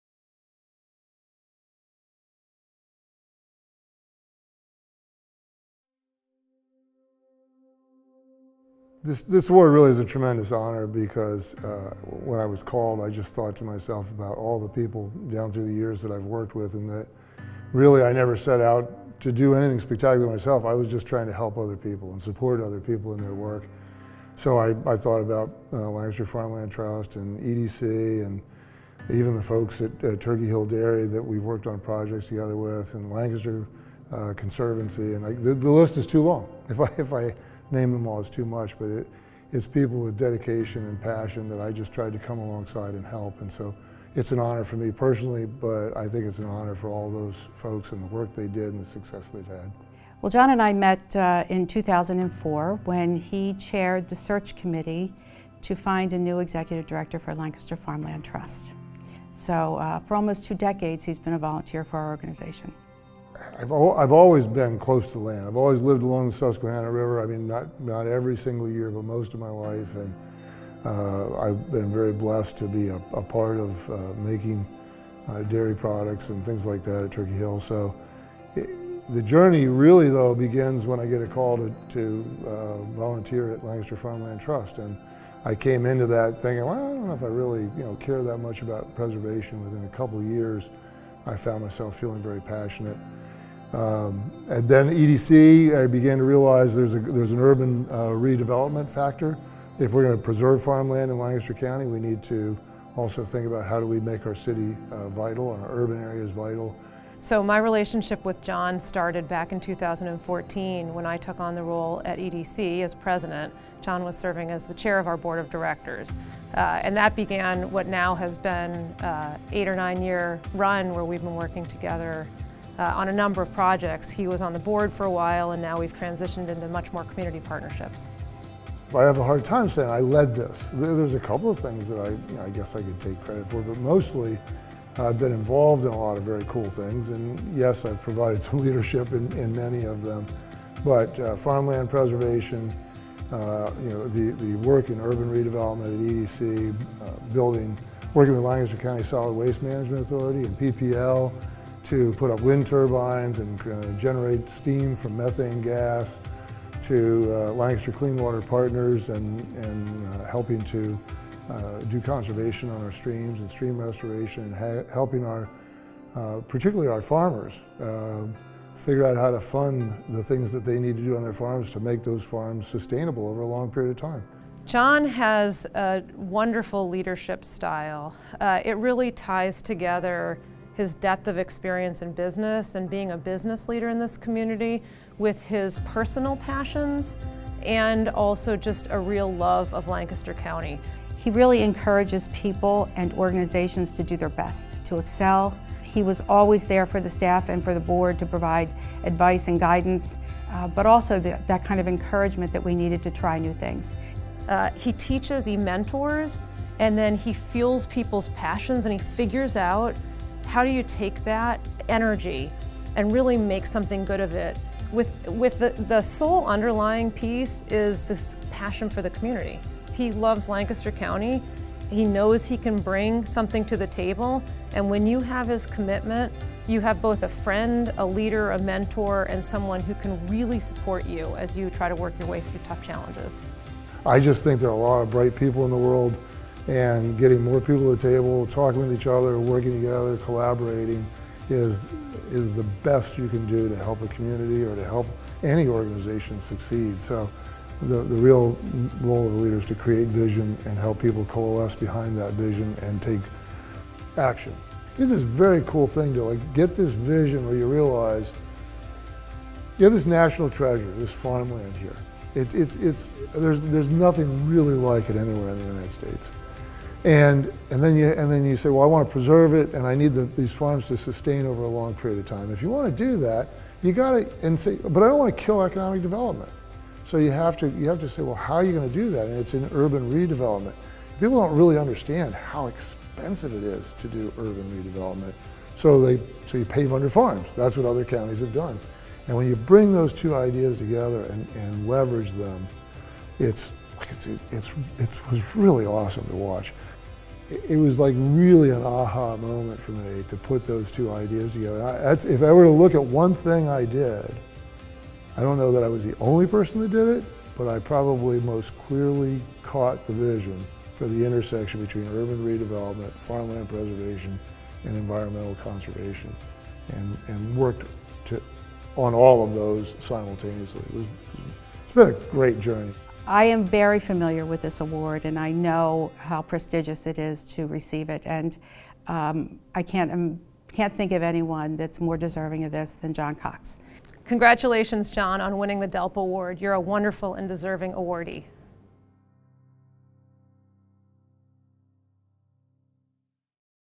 Delp Award 2021 - Lancaster Chamber
Congratulations to our 2021 George C. Delp Award honoree, John Cox.

Named for a true legend in our local agricultural history, the George C. Delp award recognizes individuals or businesses who have made significant contributions to the local agriculture industry.  From farmers to manufacturers, from government officials to bankers, the list of past recipients’ reads is as a who’s who of Lancaster agriculture.